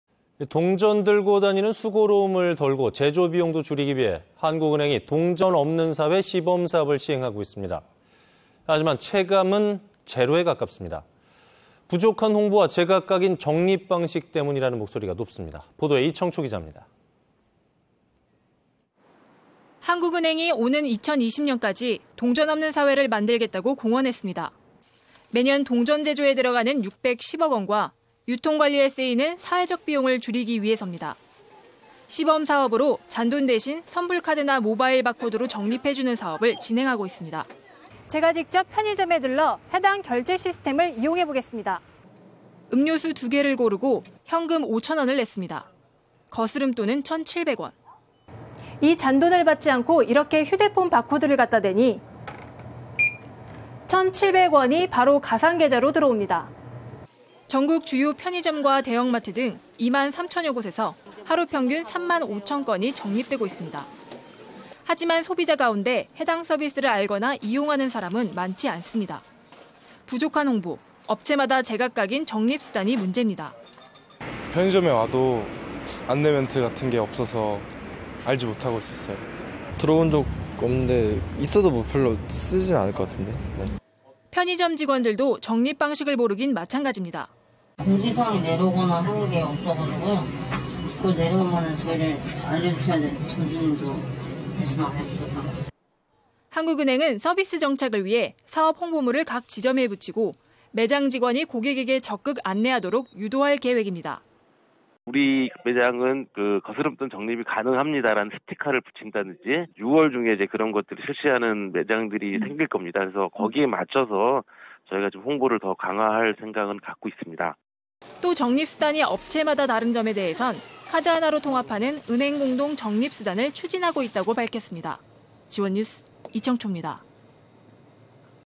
갈 길 먼 '동전없는 사회'
동전 들고다니는 수고로움을 덜고, 제조 비용도 줄이기 위해, 한국은행이 '동전 없는 사회' 시범 사업을 시행하고 있습니다.